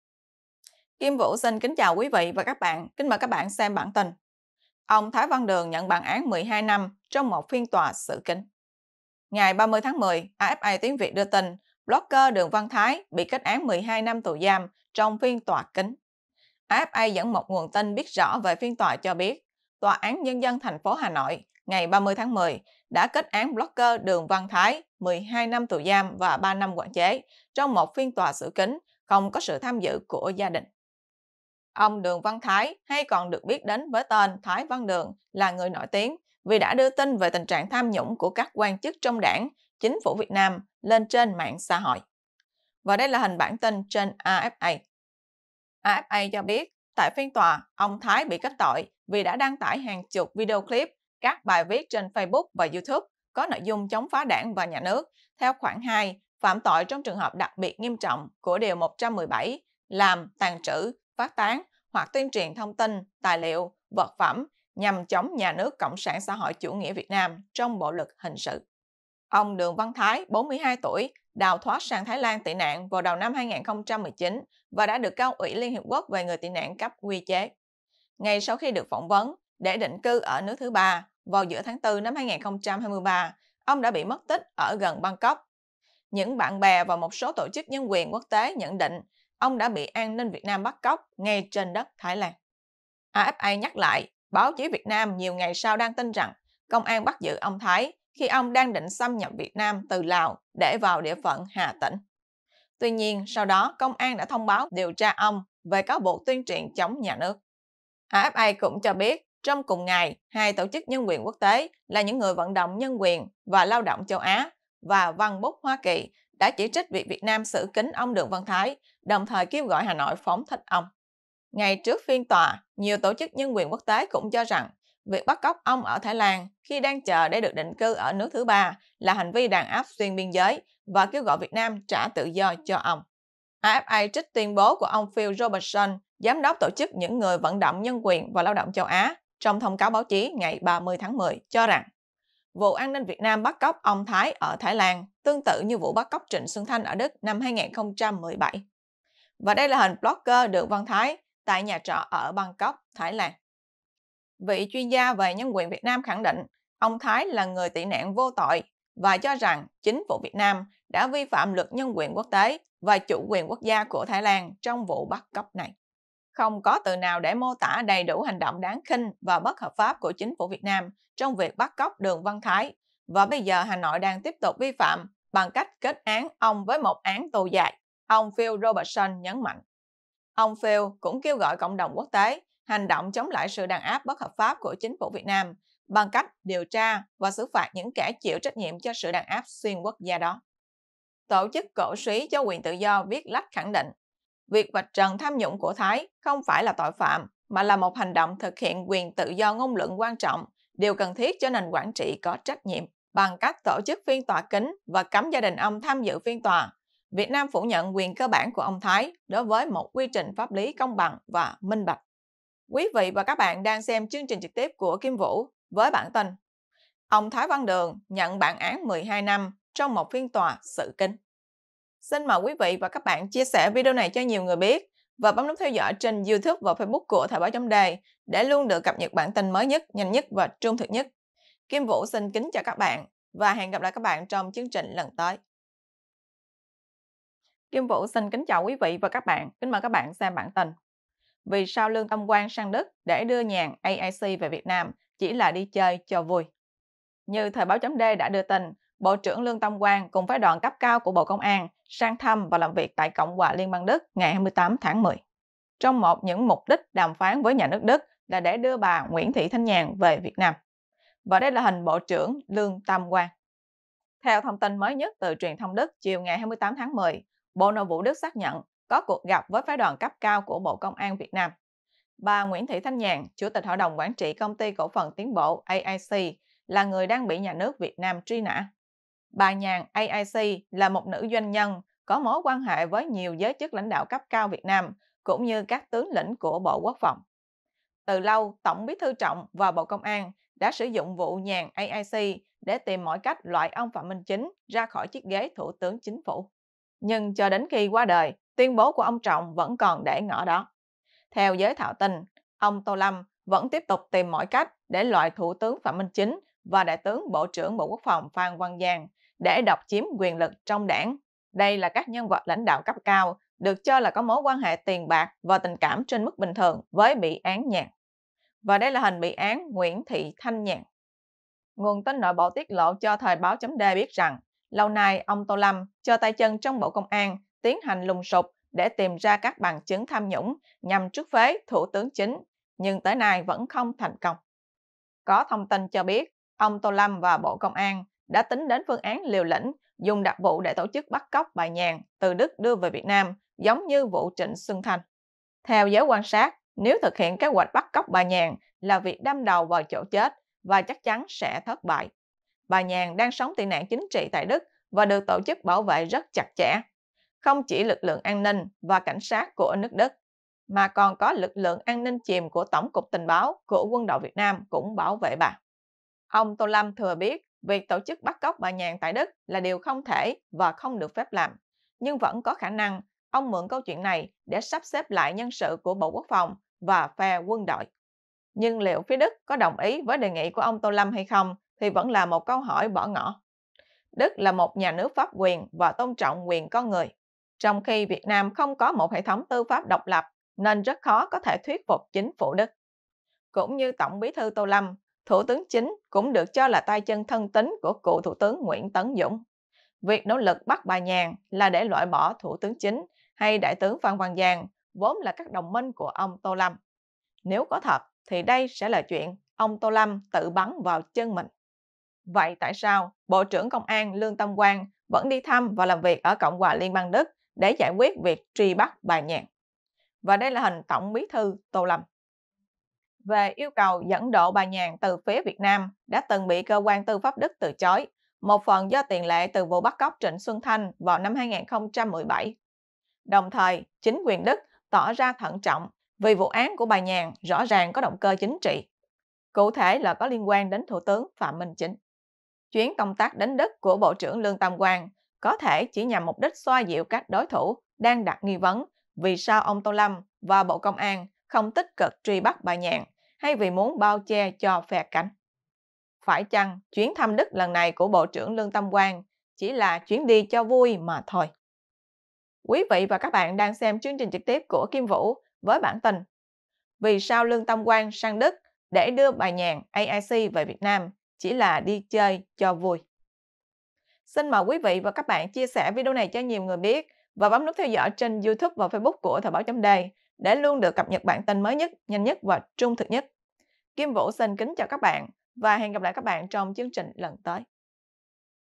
Vì sao Tô "Hốt cóc" Thái Văn Đường từ Thái Lan về Hà Nội để truy án 12 năm
Toà án Nhân dân thành phố Hà Nội ngày 30/10 đã kết án blogger Đường Văn Thái 12 năm tù giam, và 3 năm quản chế trong một phiên tòa xử kín, không có sự tham dự của gia đình.

Xem bài chi tiết tại:
Hoặc: